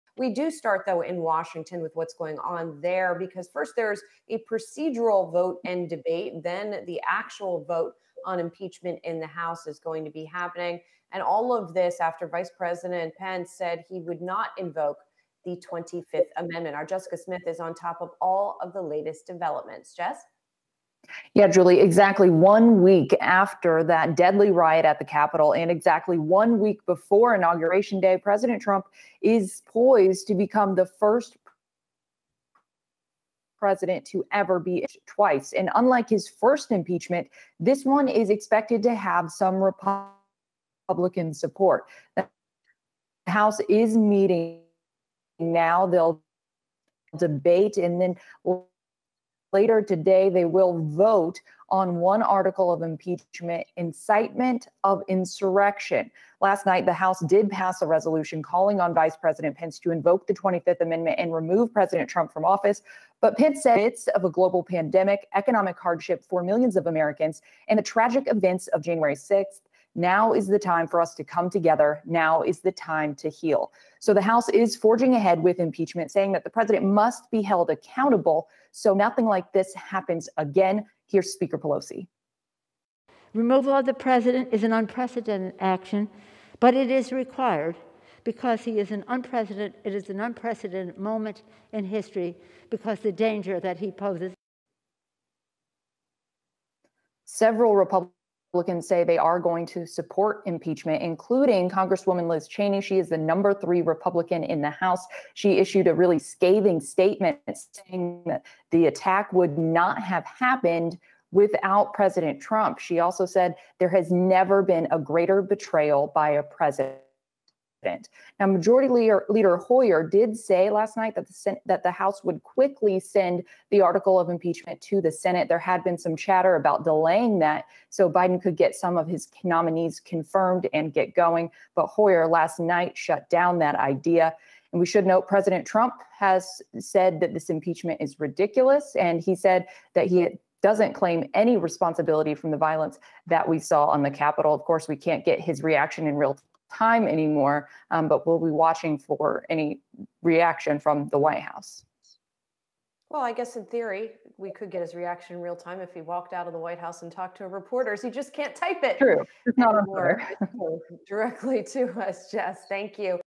VP Pence refuses to invoke the 25th Amendment, House debates and votes on 2nd impeachment
Yahoo Finance's Jessica Smith reports on the Vice President Pence's letter refusing to invoke the 25th Amendment and the House of Representatives debating and voting on on the second impeachment of President Trump.